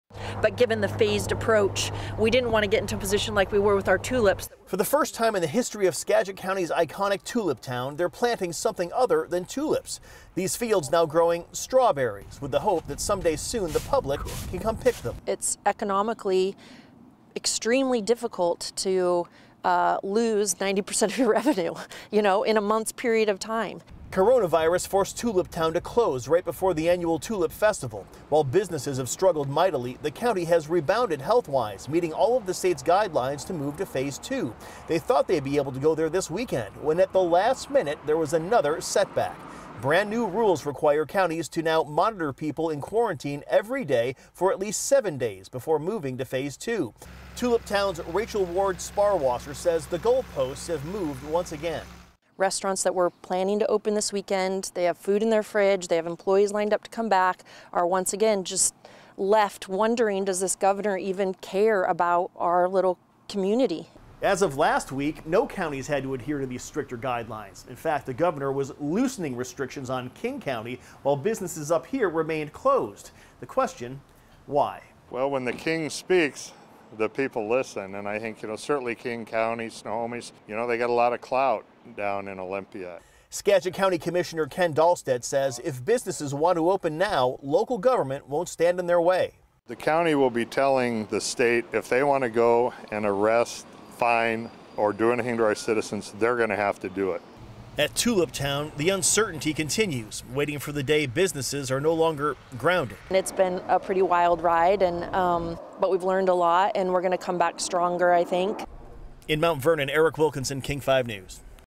Last-minute rule changes delay Skagit County's ability to move to Phase 2 of reopening
The state now wants counties to monitor those in quarantine for 7 days before they move to reopen.